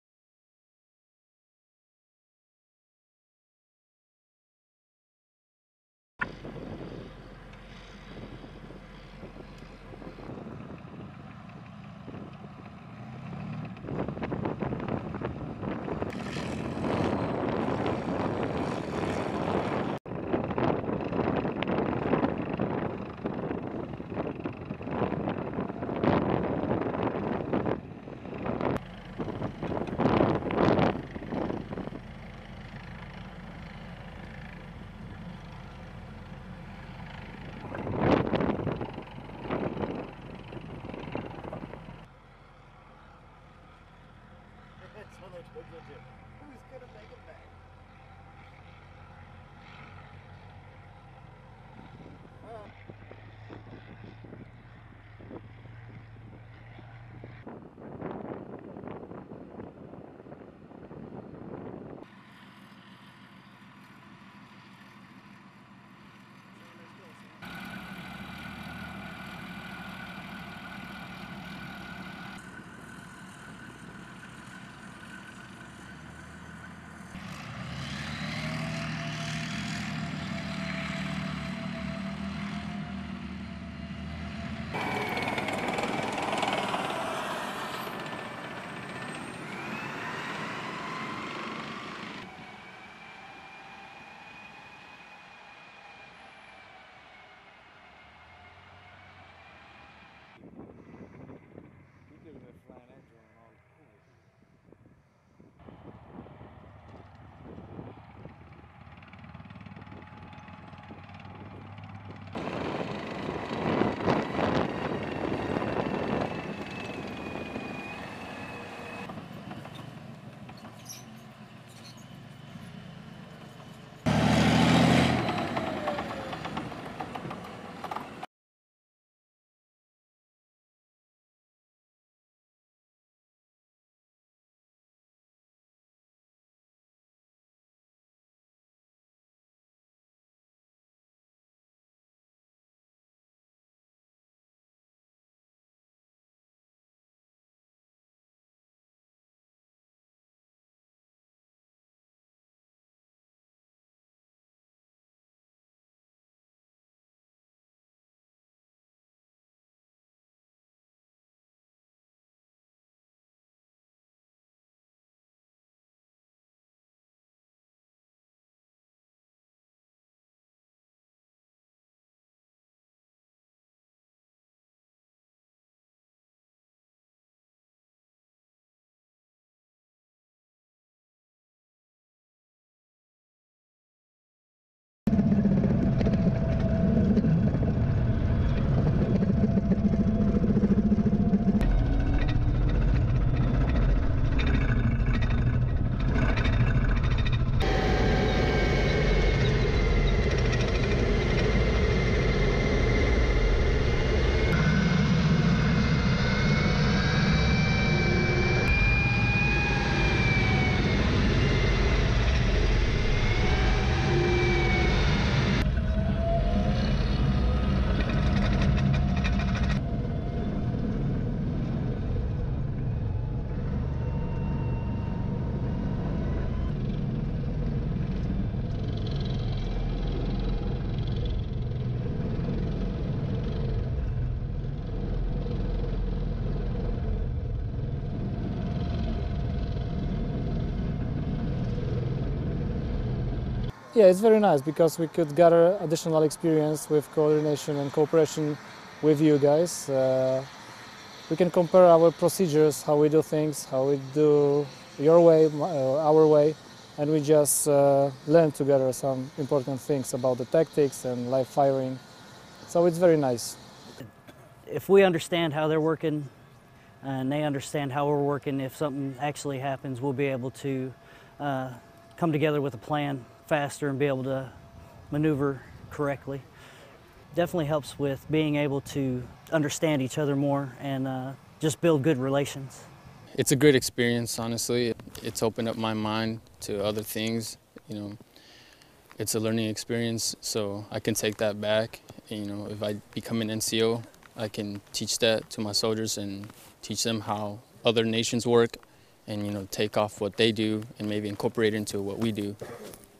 Polish Military Tanks Tactical Maneuver
Polish soldiers from the 1st Tank Battalion, 10th Armored Calvary Brigade and U.S. Soldiers from 4th Squadron, 10th Calvary Regiment, 3rd Armored Brigade Combat Team, 4th Infantry Division, come together in Karliki, Poland to improve their combined tank tactics. Report by Sgt. Austin Majors